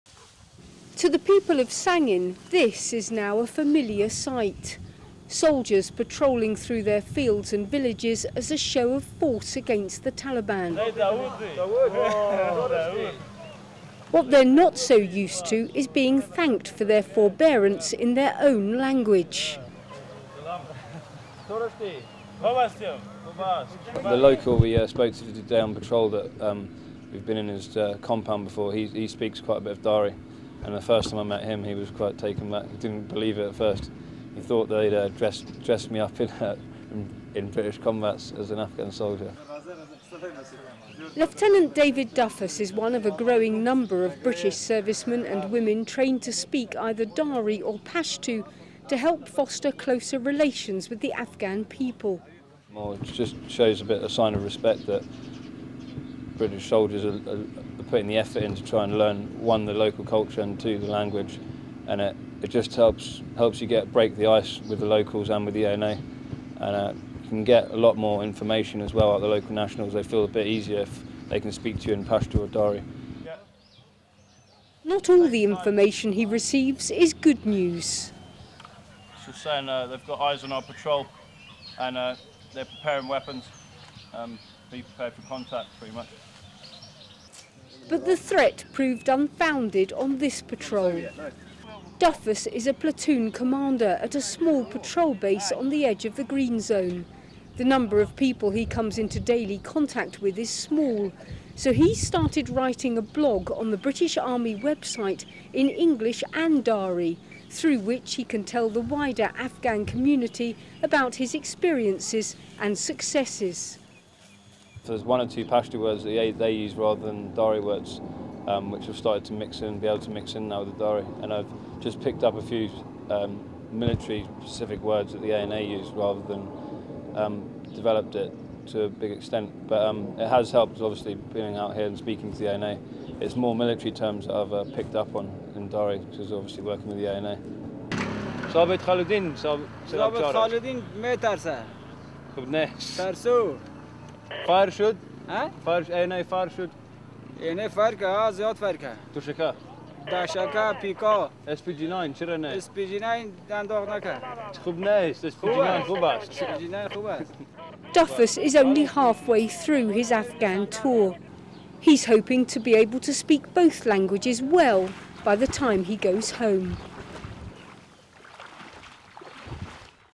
British soldiers speaking Dari
This video was filmed in summer 2010 while "embedded" with British forces in Sangin, Helmand. The British army has trained hundreds of soldiers to speak Dari and Pashtu in a bid to win hearts and minds in Afghanistan. 22 year old David Duffus is just one of those soldiers. (He also happens to play rugby for the Army First team). He wa deployed to Sangin as a platoon commander with 1 Scots from March to September 2012. He learned Dari for 9 months before his deployment. This video was made for British TV outlets to show how his language skills made his job easier in Afghanistan. It was shown at the monthly MOD brief to journalists in London and used by a number of TV channels and websites. It was devised, filmed and narrated by Sarah Yuen who was the Media Consultant to Task Force Helmand throughout 2010.